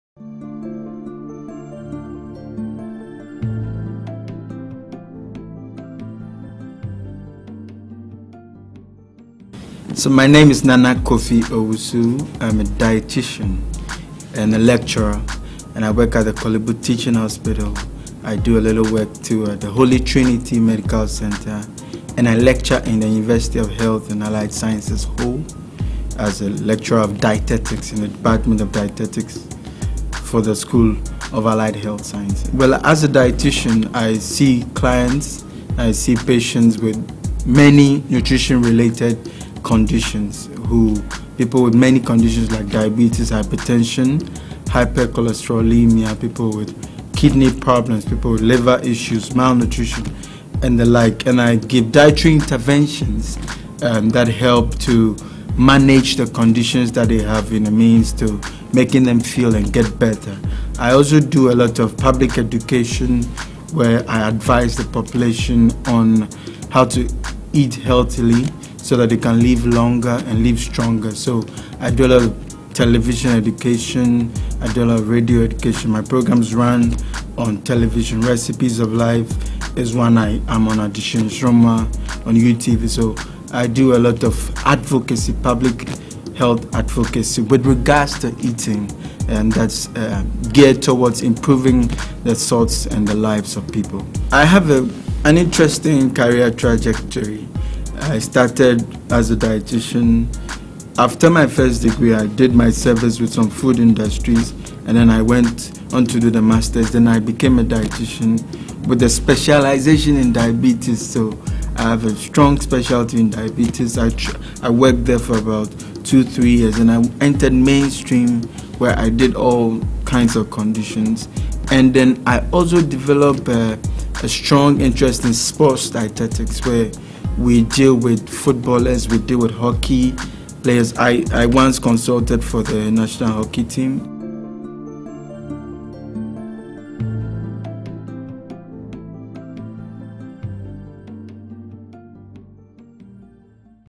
Inspire on FiestaGH | Nana Kofi Owusu Part1
A respected doctor with the Korle Bu Teaching Hospital.

He is a dietitian and a lecturer.

FiestaGH 100% Ghanaian Music, News, Entertainment and life style Channel, based in Accra, Ghana, West African. Terrestrial Digital Television on DSTV GOTV.